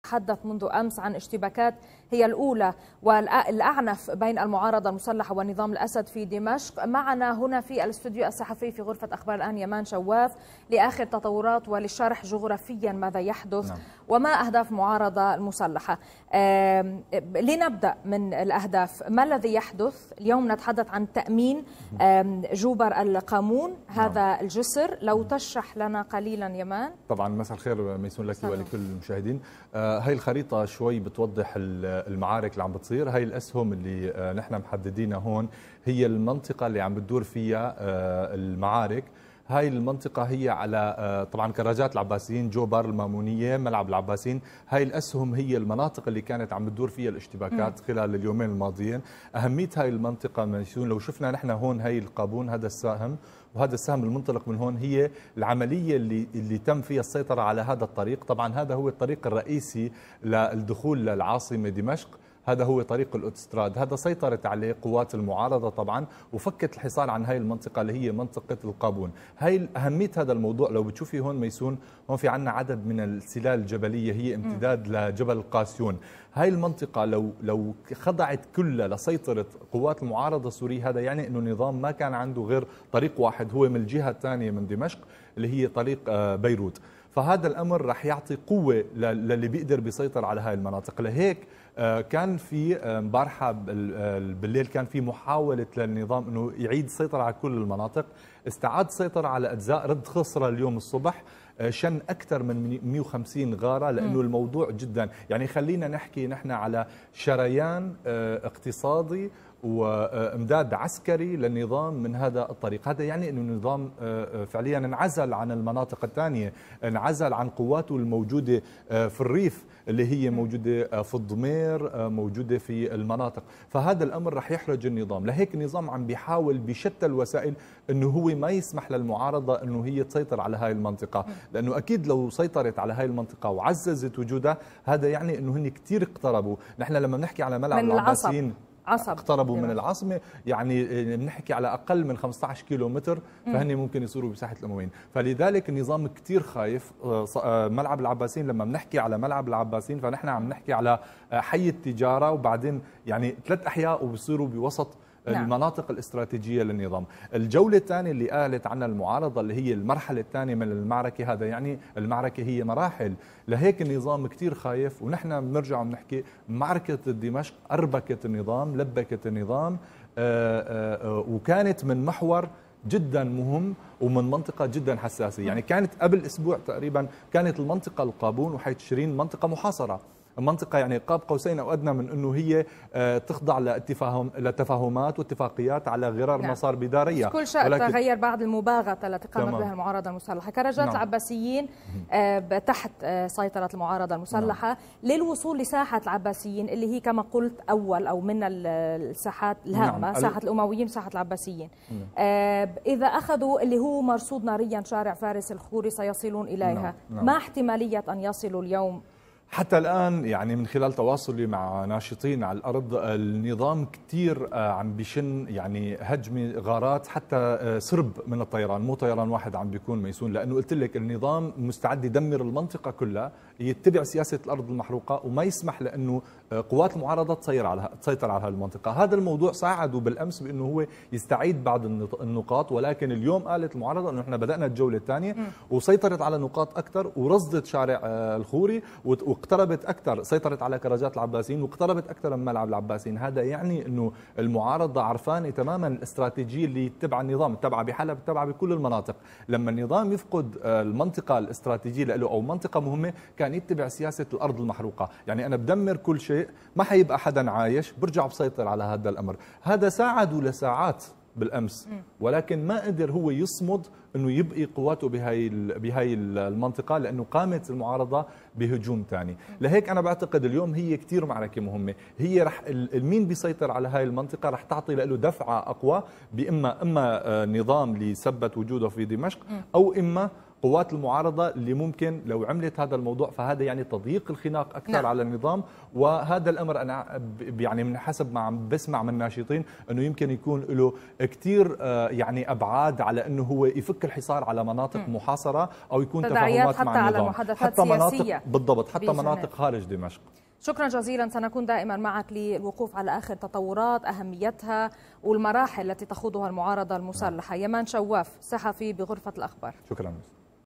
يمان شواف ومتابعة أخر تطورات المعارك قرب
شاهد آيضا:
قيادي بعثي يفضح تاريخ آل الأسد!؟
معارك كر وفر هنا على جبهتي القابون وجوبر، بالقرب من العاصمة دمشق تدور أعنف المعارك بين قوات نظام_الأسد وقوات المعارضة_السورية، حيث نجحت قوات المعارضة من مباغتت قوات نظام الأسد والسيطرة على مواقع إستراتيجية في حيي جوبر والقابون.

محمد الطويل مقاتل يرصد جبهة الأوتستراد الدولي في حي القابون " نحن هنا في حي القابون نظام الأسد يحاول اقتحام الحي وإستعادة المناطق التي خسرها لكننا صامدون هنا، نتمنى ونطلب من كل الجبهات بدأ المعارك ضد نظام الأسد، نحن نفعل ما علينا ويجب عليكم أن تساعدونا أيضاً".

تقديم: ميسون بركة March 2017